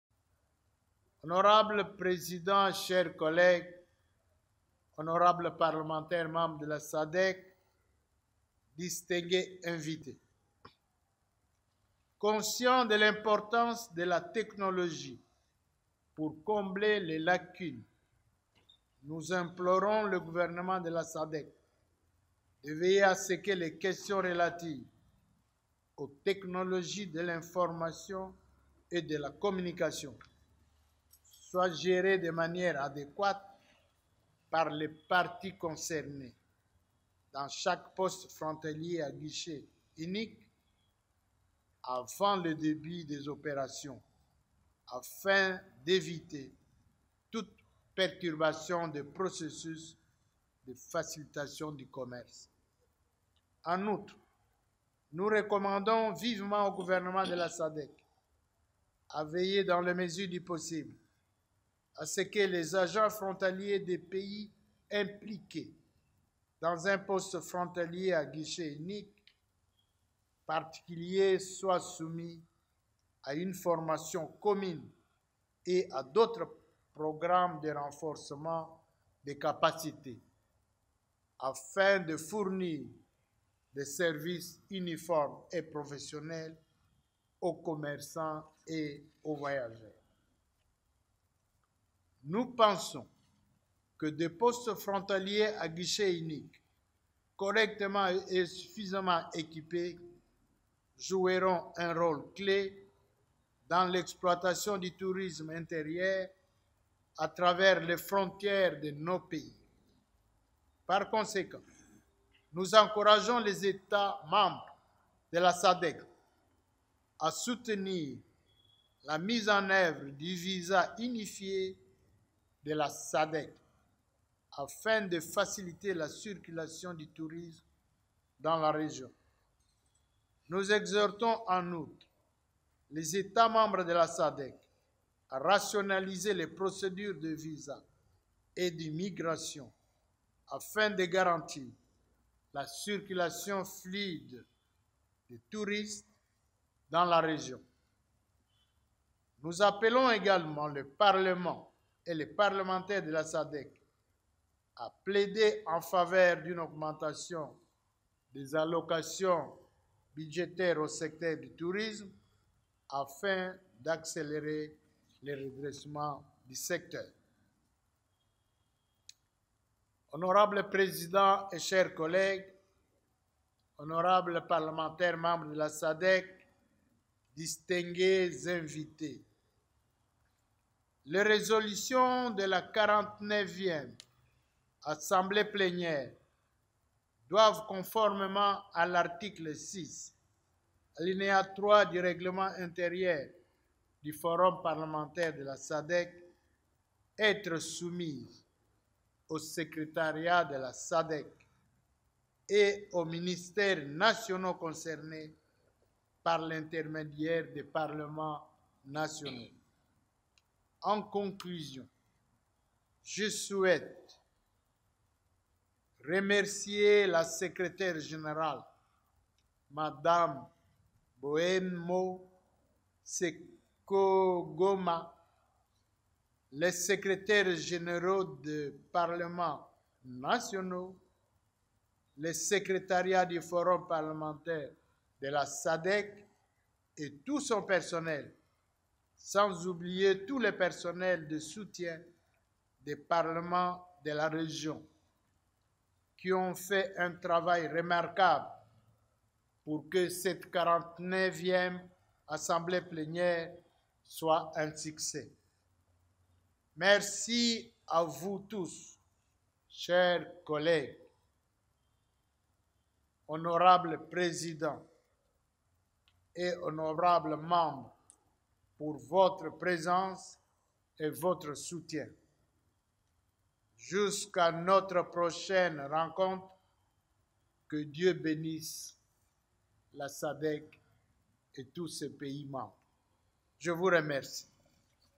RDC: ASS. NAT NOUS APPELONS LES ETATS MEMBRES DE LA SADEC A ELIMINER LE MUR INVISIBLE QUI ENTRAVE...
Video from Mosakoli Shako